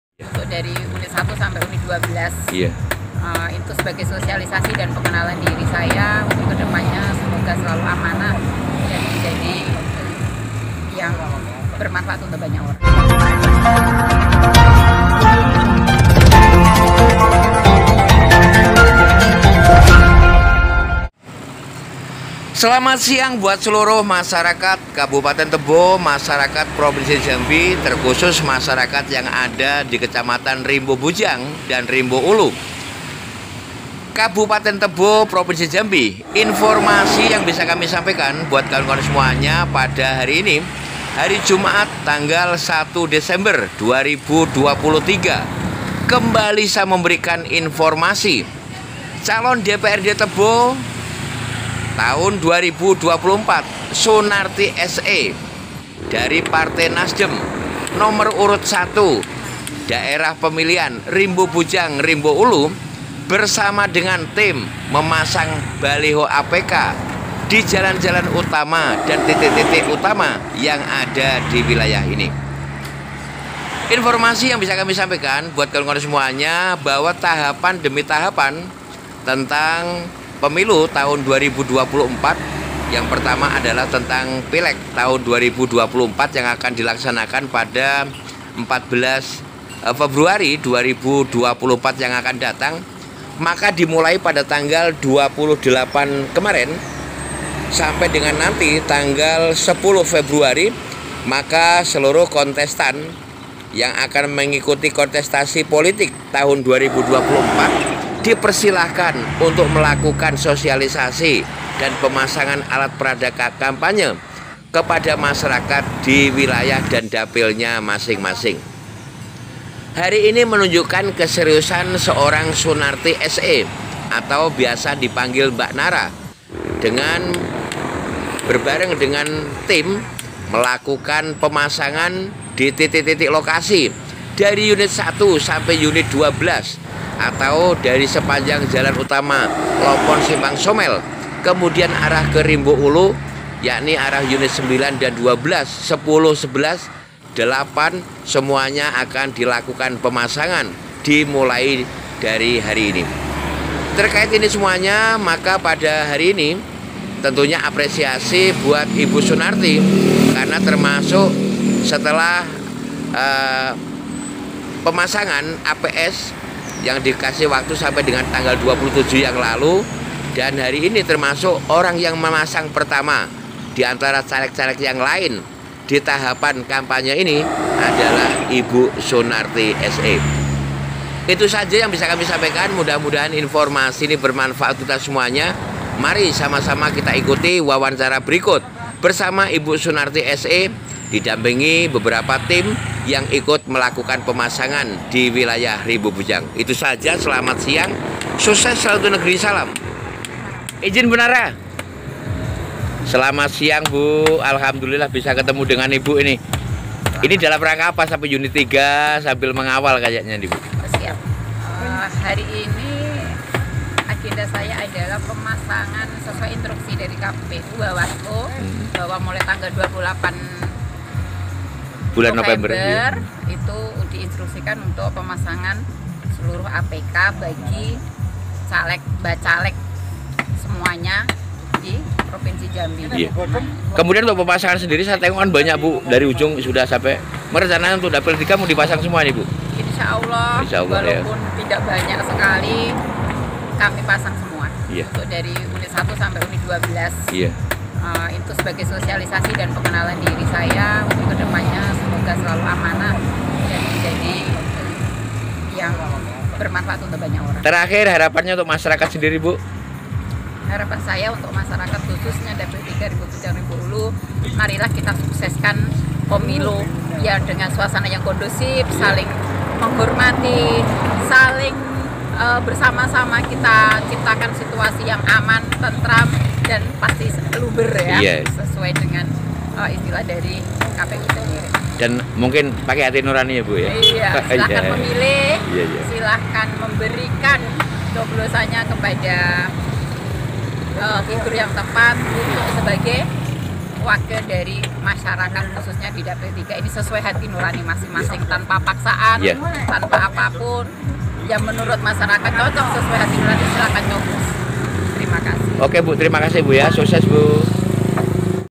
SUNARTI,SE Calon DPRD tebo 2024 Nasdem Dapil 3(Rimbo bujang Rimbo ulu)nomor1 bersama Tim pasang APK
Jumat (1/12/2023)